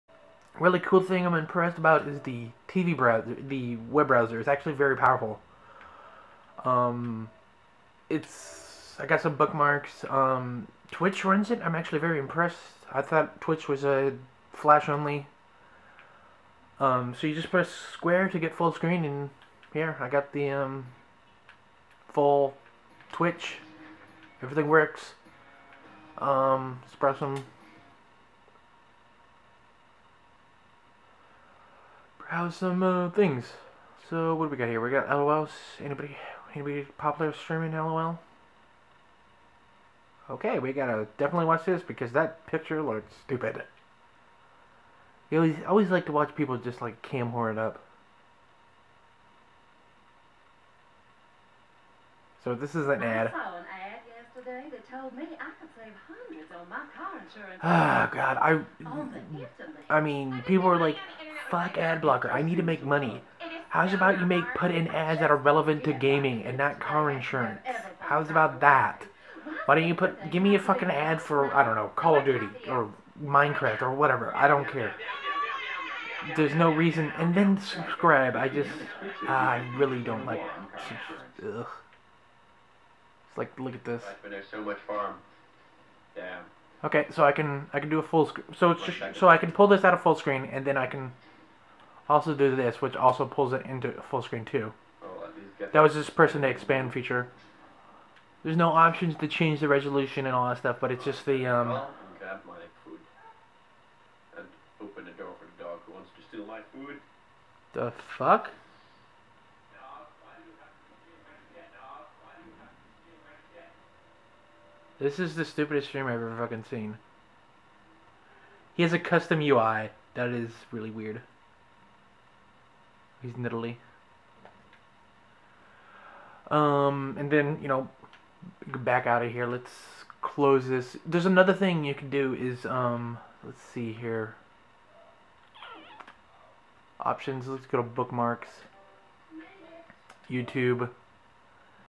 PS4 Web Browser
A quick go through of the web browser built into the PS4. Thought the PS4 doesn't have a  Youtube app. Going to youtube.tv works as the PS3 version.